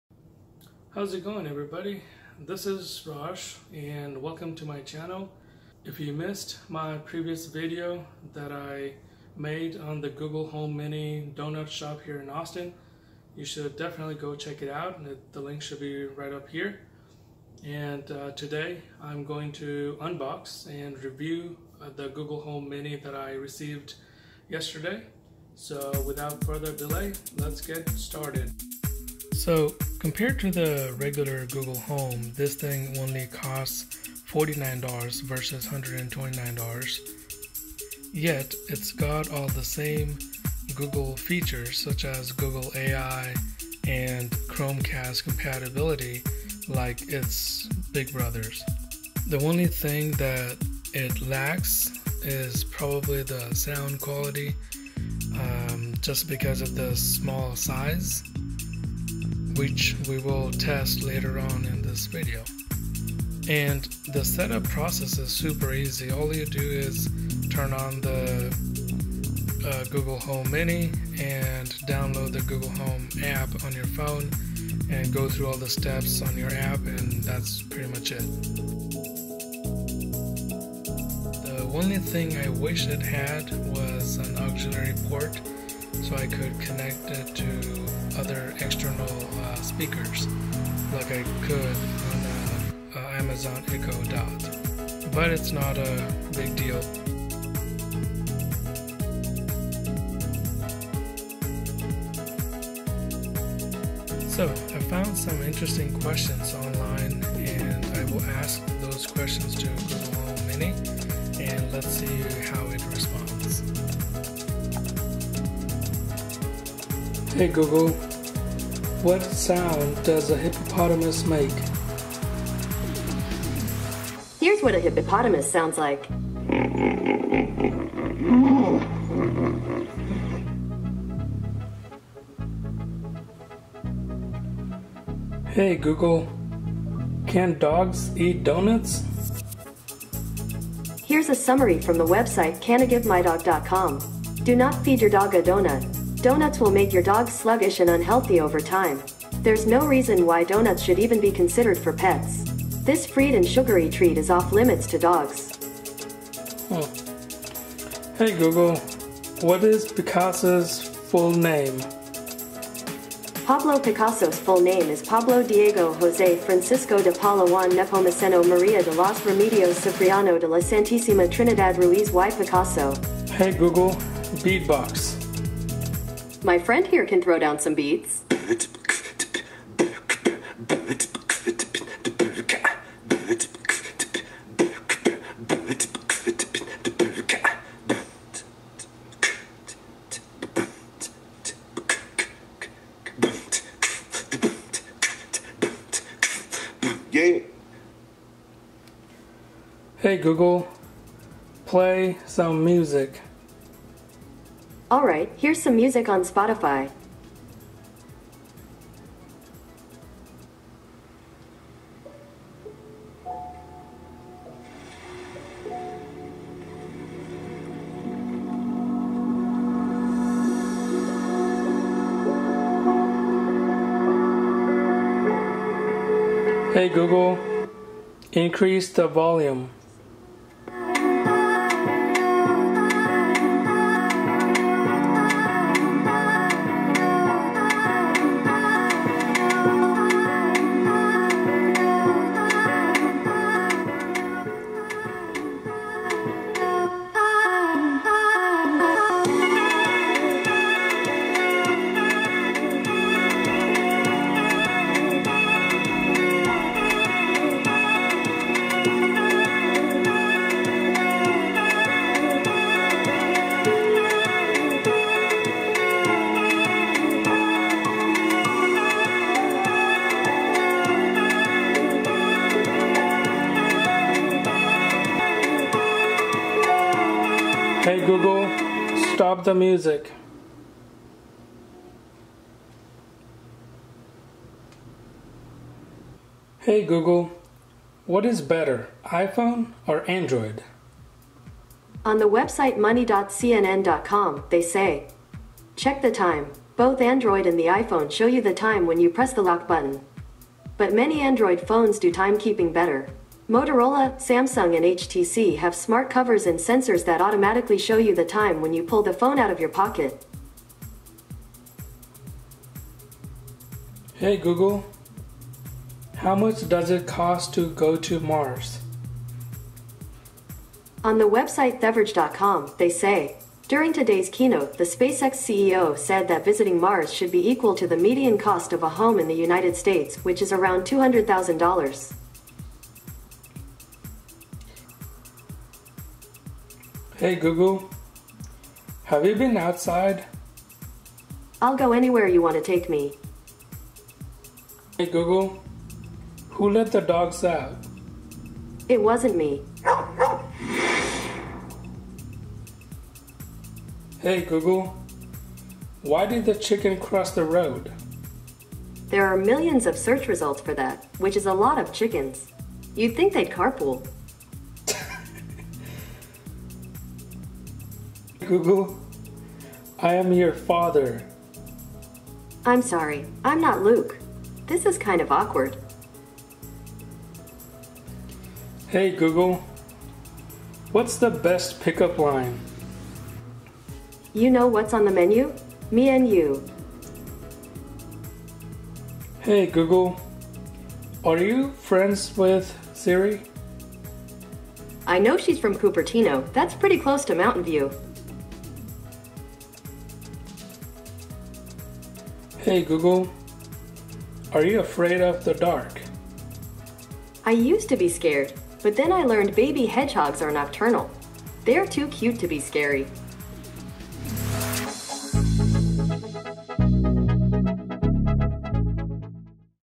Google Home Mini Quick Set up and Review
This is a quick set up and review video on the Google Home Mini speaker. I have only covered a small part of all the features that this speaker/ digital assistant has to offer. Enjoy..